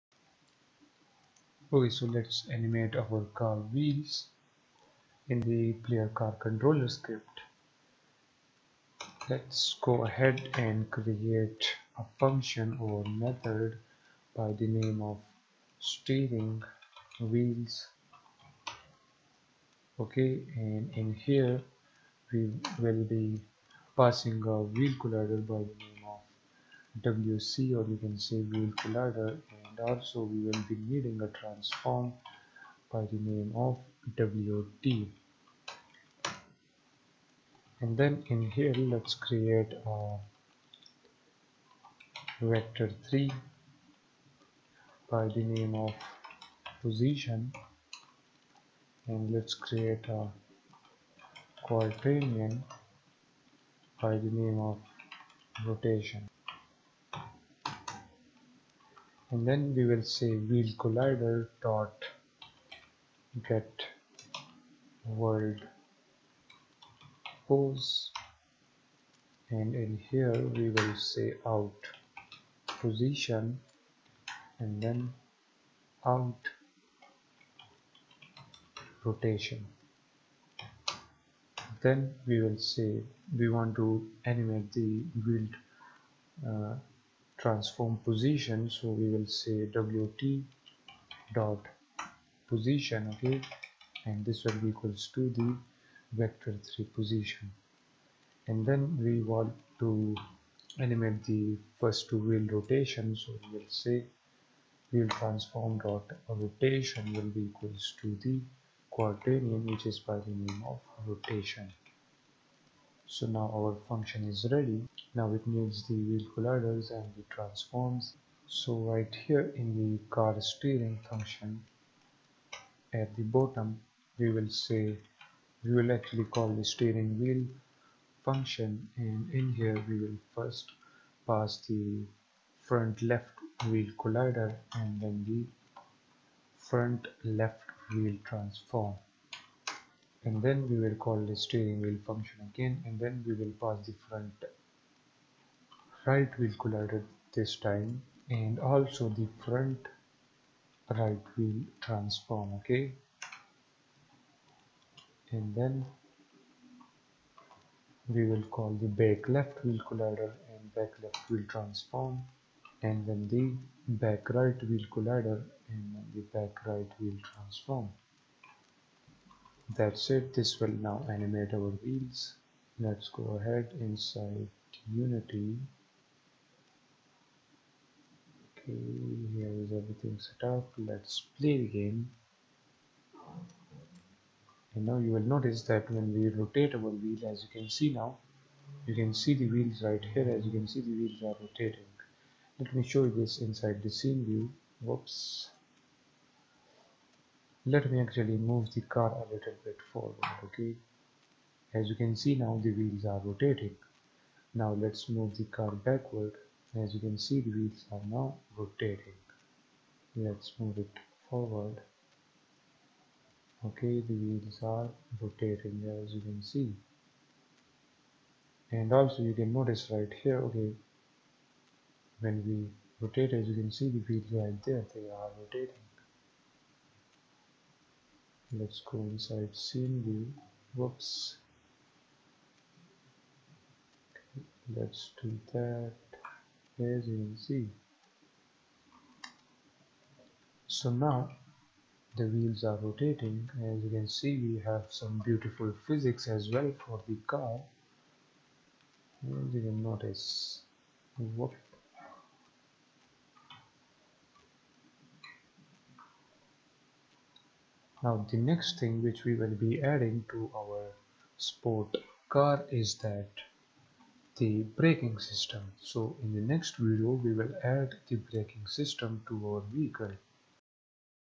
Realistic Car Controller Unity 3D Android & iOS Game Development - Car Wheel Animation & Rotation
This is tutorial#10 of this course. In this course you will learn how to make android game in unity game engine & how to make ios game in unity game engine by building 3d car racing game, the fast and furious city car racing 3d game. So step by step you will learn 3d mobile game development and at the end of this course you will be able to make your own 3d android & ios games using unity game engine.